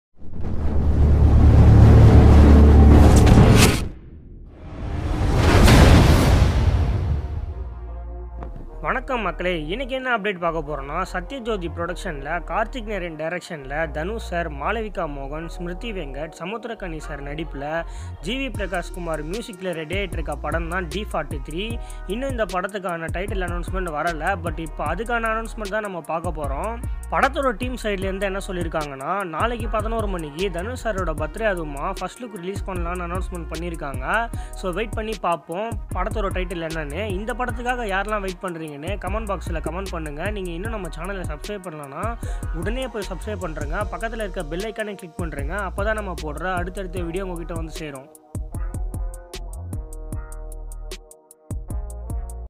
D43 Official fist look | Dhanush | Karthik narean | Malavika mohan | GV Prakash | BWC
This video about dhanush next movie d43 first look update.